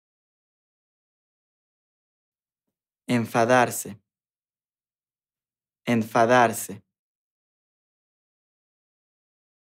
How to pronounce ENFADARSE in Latin American Spanish
This video shows you how to pronounce ENFADARSE in Latin American Spanish. Speaker has an accent from Queretaro (central Mexico) . Collins Dictionary, the home of living language and pioneers of dictionary publishing